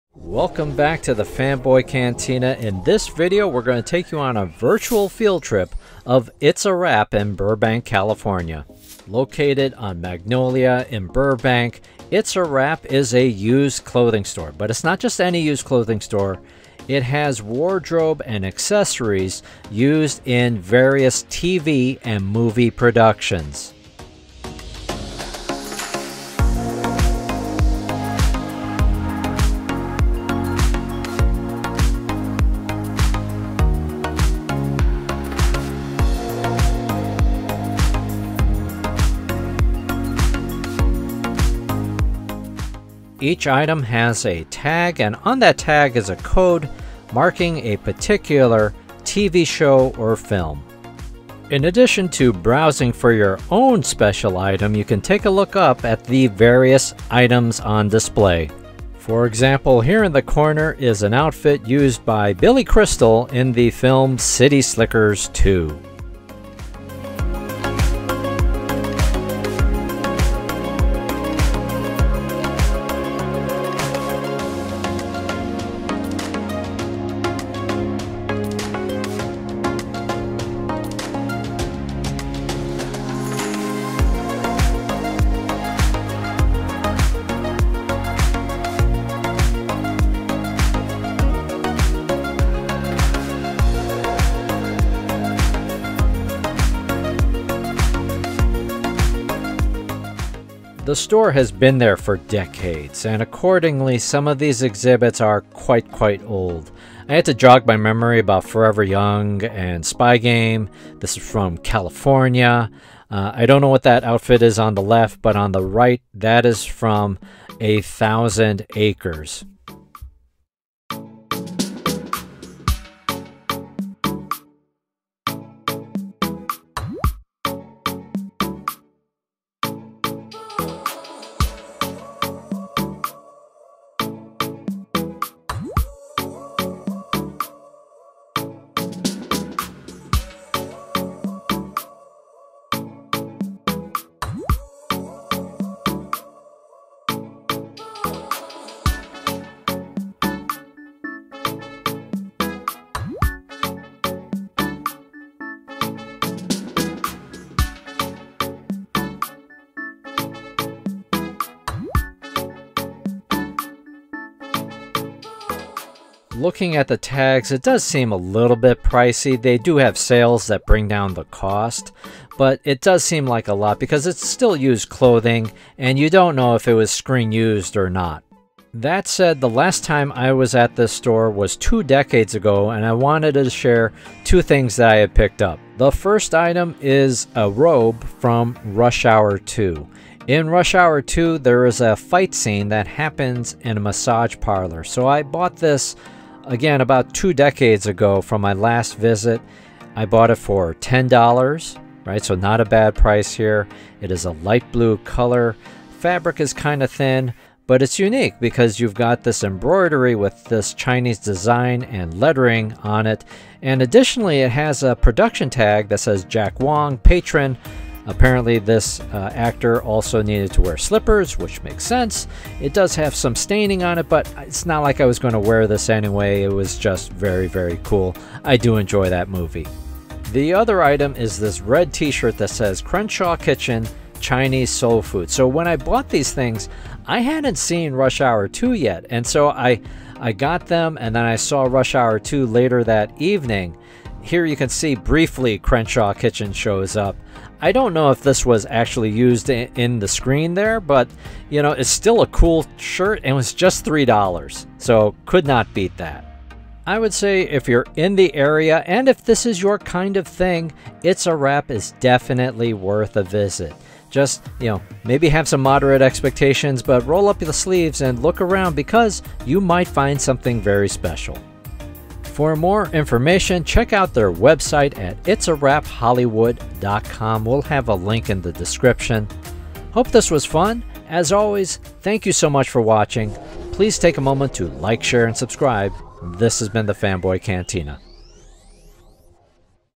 It's a Wrap | SoCal Thrift Store for Used TV/Movie Production Wardrobe | Virtual Field Trip
Join our virtual field trip to It's a Wrap in Burbank, CA! This unique store sells used clothing and accessories, but not just any used clothing ... the store specializes in TV and movie production wardrobe sales. Be sure to watch to the end to see two items that I purchased back in 2001 from the film Rush Hour 2.

You can show your support for Fanboy Cantina with the latest merch available from Wistful on Amazon, such as this shirt